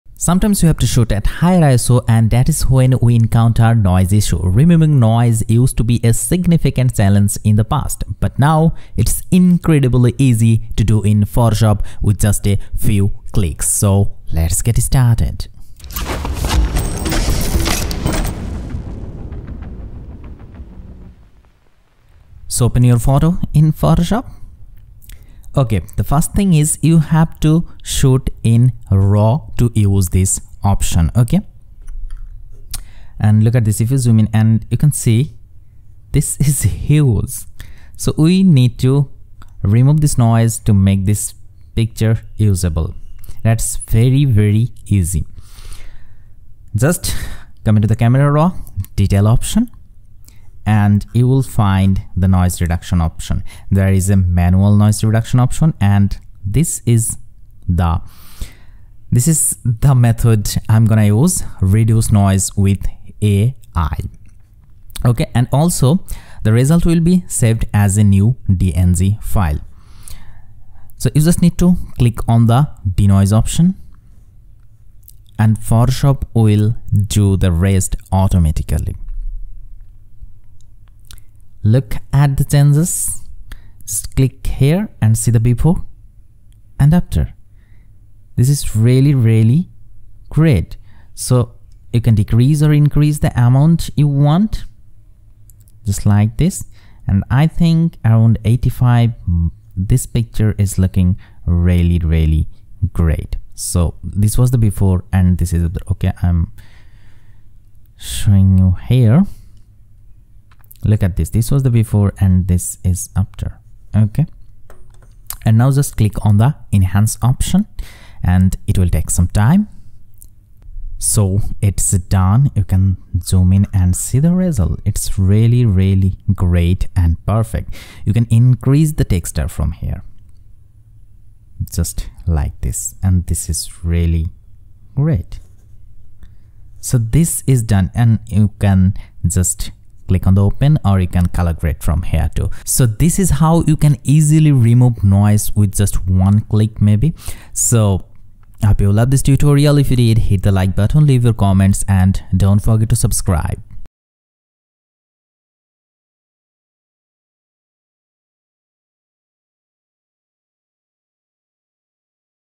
REMOVE NOISE IN ONE CLICK - PHOTOSHOP AI
Hi, welcome to Farzzology, In this quick video I am gonna show you how to remove noise in photoshop easily !!
Hope you will love this.